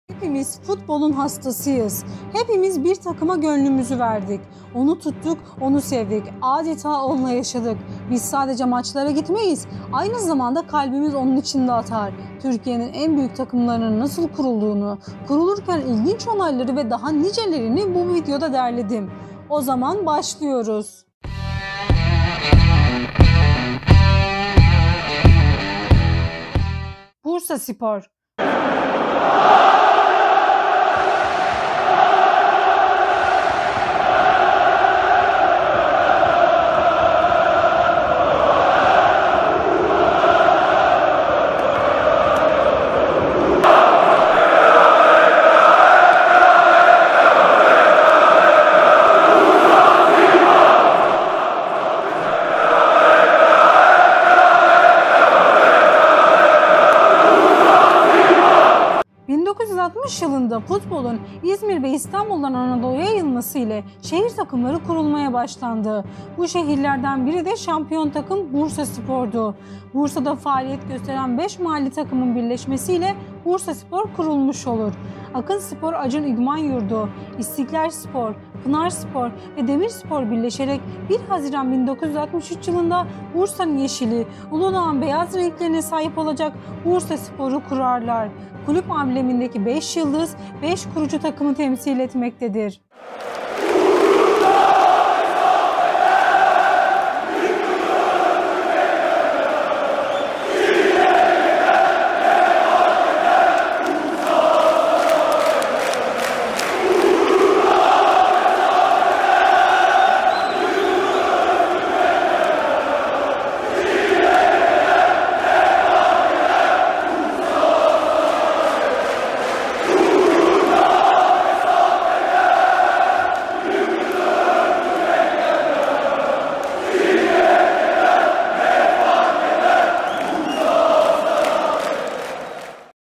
Kuruluş: Bursaspor'un Kuruluş Hikayesi ve Tarihi. Bursaspor Nasıl Kuruldu?
1960 yılında futbolun İzmir ve İstanbul’dan Anadoluya yayılması ile şehir takımları kurulmaya başlandı. Bu şehirlerden biri de Şampiyon takım Bursaspor’du. Bursa’da faaliyet gösteren 5 mahalli takımın birleşmesiyle Bursaspor kurulmuş olur. Akınspor Acun İdman Yudu, İstiklalspor, Pınarspor ve Demirspor birşeleşerek 1 Haziran 1963 yılında Bursanın yeşili Uludağın beyaz renklerine sahip olacak şekilde Bursasporu kurarlar. Kulüp amblemindeki 5 yıldız, 5 kurucu takımı temsil etmektedir.